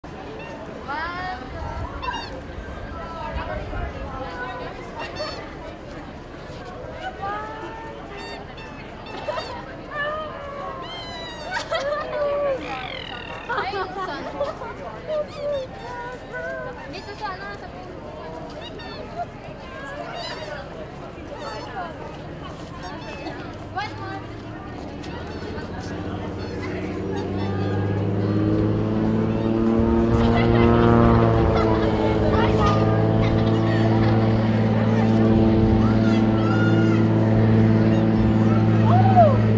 Girls want more than one picture with Kheetah! - 26
Kheetah Fursuiting at Canada Place Vancouver - 26
Kheetah wanders around a nearby Anime convention to see what he can find!Filmed in Aug 2012